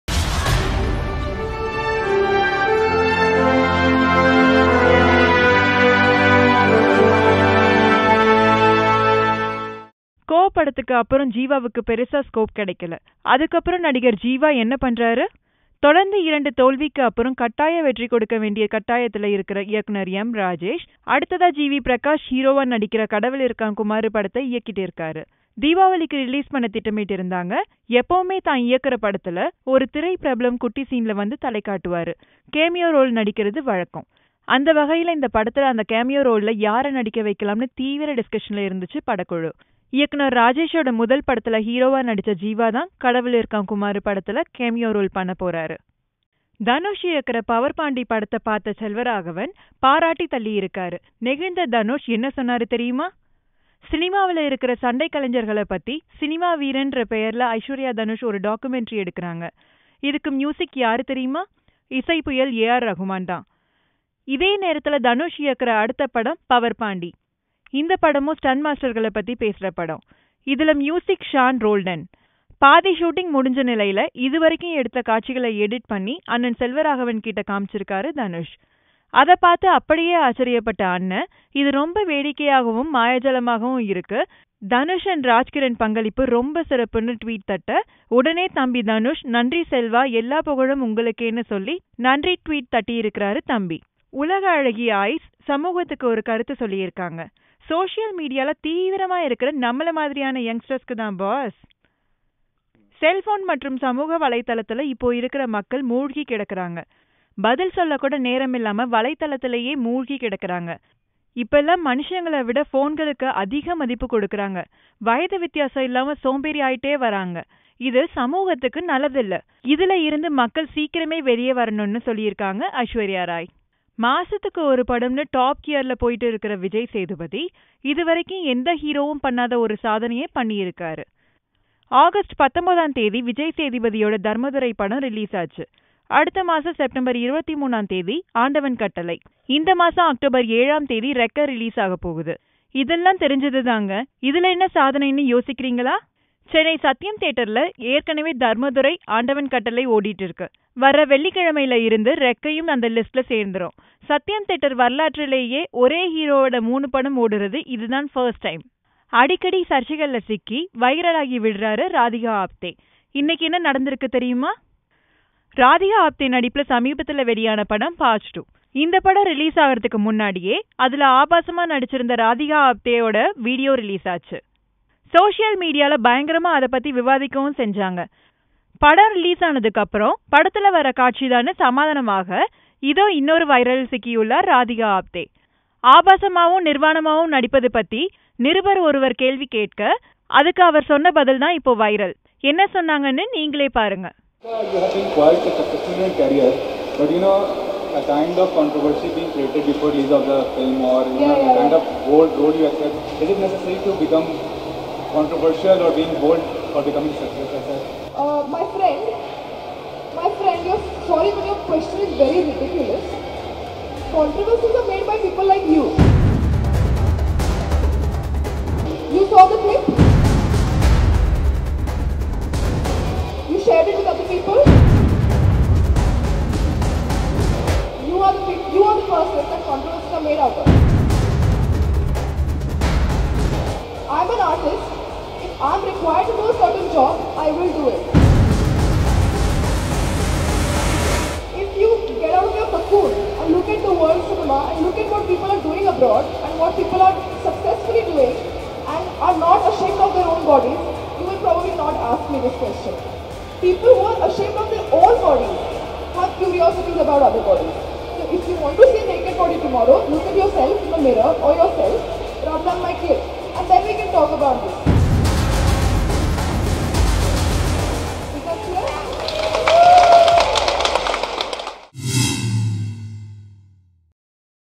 Not Ashamed Of Anything: Radhika Apte On Leaked Parched Video
Not Ashamed Of Anything: Radhika Apte On Leaked Parched Video For more videos